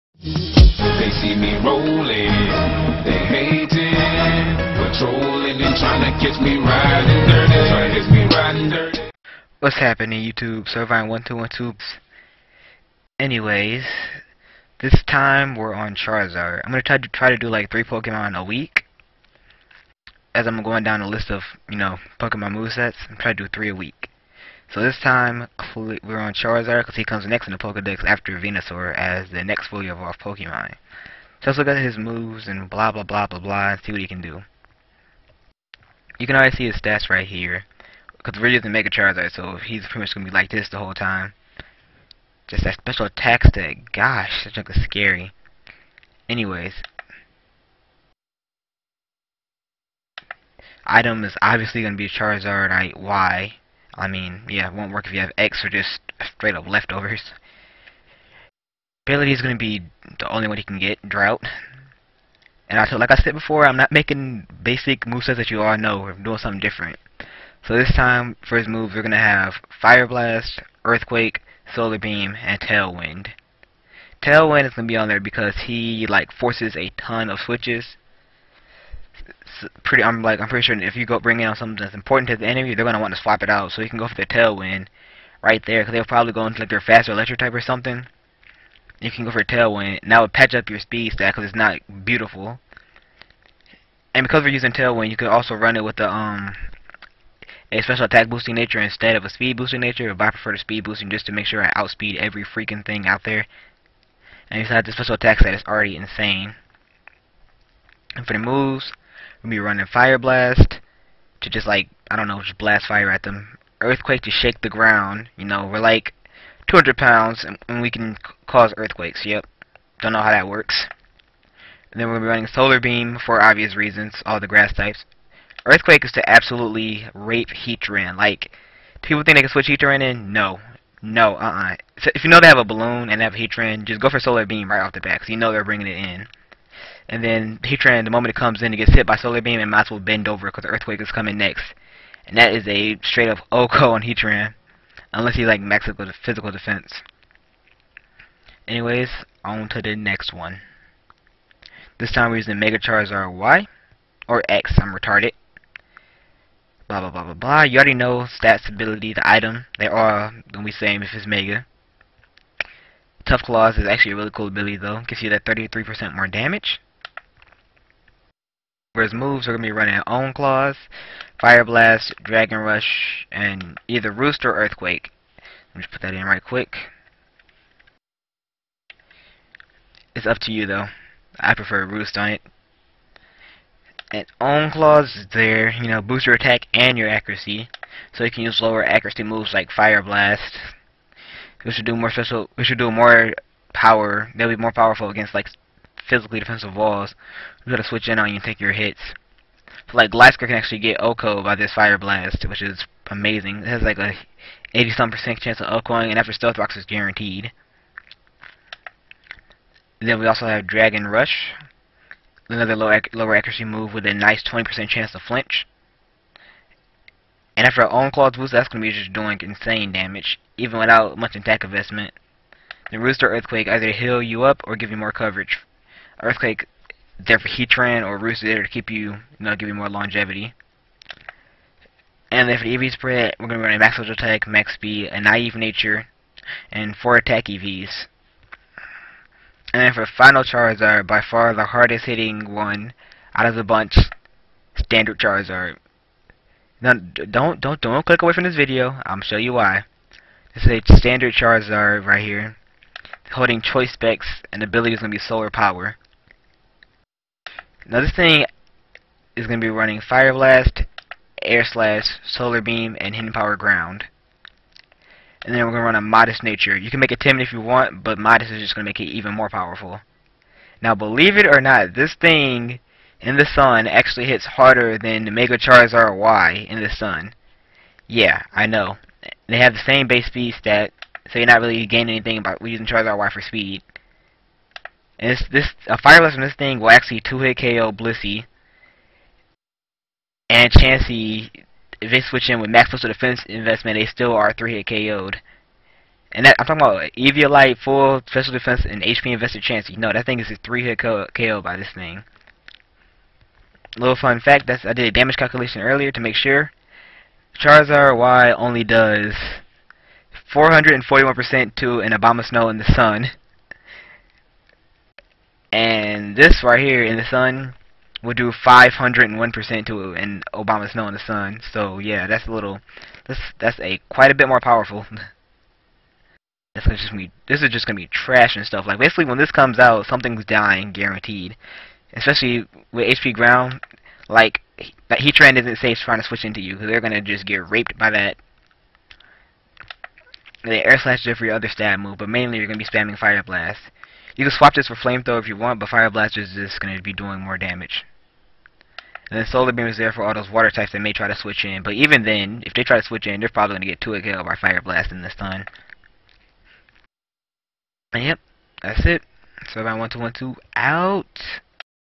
Charizard Movesets
Charizard Has some pretty standard sets so here are some unique movesets.